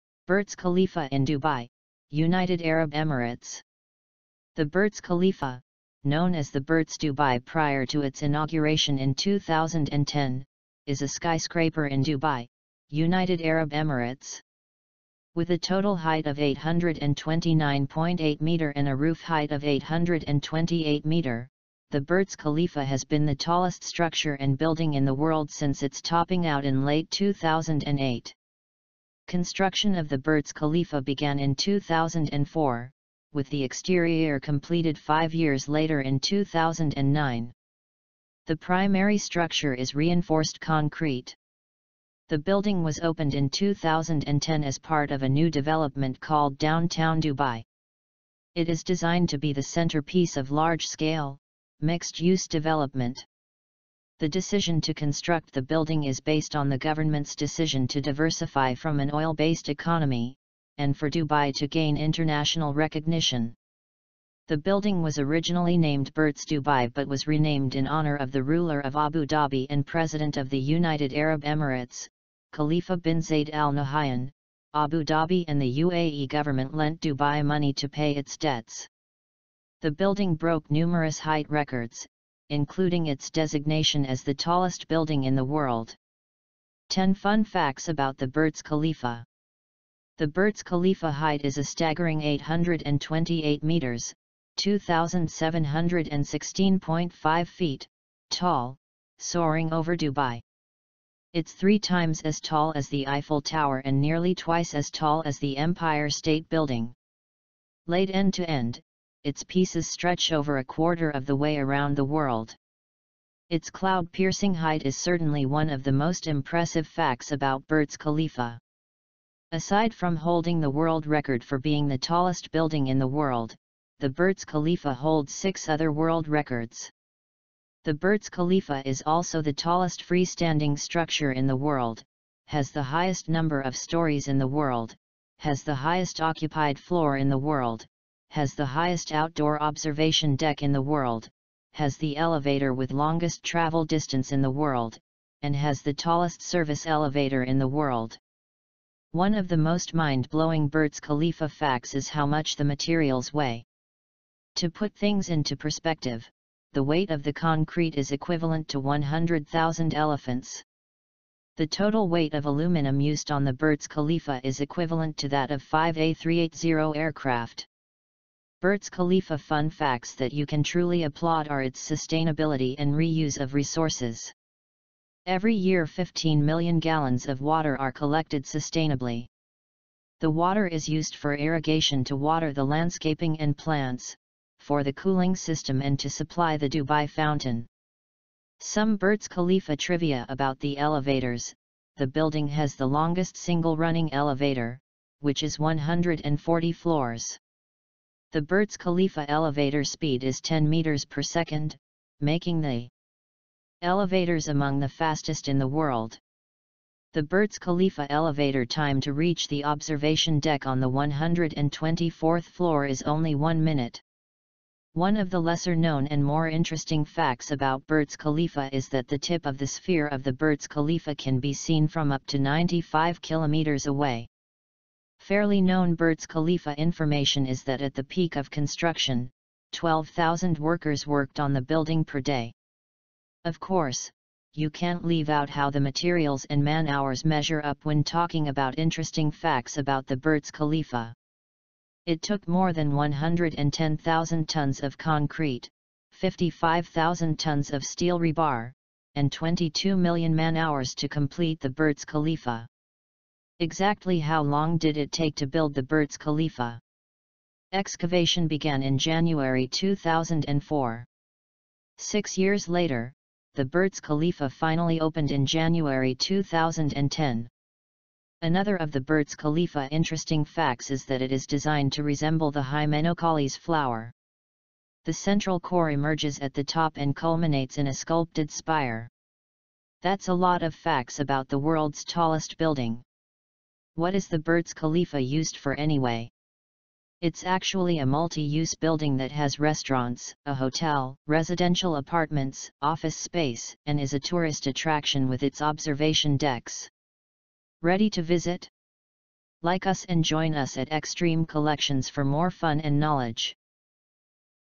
Burj Khalifa in Dubai‎, United Arab Emirates
The Burj Khalifa, known as the Burj Dubai prior to its inauguration in 2010, is a skyscraper in Dubai, United Arab Emirates. With a total height of 829.8 m and a roof height of 828 m, the Burj Khalifa has been the tallest structure and building in the world since its topping out in late 2008.

Construction of the Burj Khalifa began in 2004, with the exterior completed five years later in 2009. The primary structure is reinforced concrete. The building was opened in 2010 as part of a new development called Downtown Dubai. It is designed to be the centrepiece of large-scale, mixed-use development. The decision to construct the building is based on the government's decision to diversify from an oil-based economy, and for Dubai to gain international recognition. The building was originally named Burj Dubai but was renamed in honour of the ruler of Abu Dhabi and president of the United Arab Emirates, Khalifa bin Zayed Al Nahyan; Abu Dhabi and the UAE government lent Dubai money to pay its debts. The building broke numerous height records, including its designation as the tallest building in the world.

10 Fun Facts about the Burj Khalifa
• The Burj Khalifa height is a staggering 828 meters (2716.5 feet) tall, soaring over Dubai. It’s three times as tall as the Eiffel Tower and nearly twice as tall as the Empire State Building. Laid end to end, its pieces stretch over a quarter of the way around the world. It’s cloud-piercing height is certainly one of the most impressive facts about Burj Khalifa.

• Aside from holding the world record for being the tallest building in the world, the Burj Khalifa holds six other world records. The Burj Khalifa is also the tallest freestanding structure in the world, has the highest number of stories in the world, has the highest occupied floor in the world, has the highest outdoor observation deck in the world, has the elevator with longest travel distance in the world, and has the tallest service elevator in the world.

• One of the most mind blowing Burj Khalifa facts is how much the materials weigh. To put things in to perspective, the weight of the concrete is equivalent to 100,000 elephants. The total weight of aluminum used on the Burj Khalifa is equivalent to that of five A380 aircraft.

• Burj Khalifa fun facts that you can truly applaud are its sustainability and reuse of resources. Every year 15 million gallons of water are collected sustainably. The water is used for irrigation to water the landscaping and plants, for the cooling system and to supply the Dubai Fountain.

• Some Burj Khalifa trivia about the elevators: the building has the longest single running elevator, which is 140 floors. The Burj Khalifa elevator speed is 10 meters per second, making the
elevators among the fastest in the world. The Burj Khalifa elevator time to reach the observation deck on the 124th floor is only one minute.

• One of the lesser known and more interesting facts about Burj Khalifa is that the tip of the sphere of the Burj Khalifa can be seen from up to 95 kilometers away.

• Fairly known Burj Khalifa information is that at the peak of construction, 12,000 workers worked on the building per day.

• Of course, you can’t leave out how the materials and man hours measure up when talking about interesting facts about the Burj Khalifa. It took more than 110,000 tons of concrete, 55,000 tons of steel rebar, and 22 million man-hours to complete the Burj Khalifa.

• Exactly how long did it take to build the Burj Khalifa? Excavation began in January 2004. Six years later, the Burj Khalifa finally opened in January 2010.

• Another of the Burj Khalifa interesting facts is that it is designed to resemble the Hymenocallis flower. The central core emerges at the top and culminates in a sculpted spire.

That’s a lot of facts about the world’s tallest building. What is the Burj Khalifa used for anyway? It’s actually a multi-use building that has restaurants, a hotel, residential apartments, office space and is a tourist attraction with its observation decks. Ready to visit?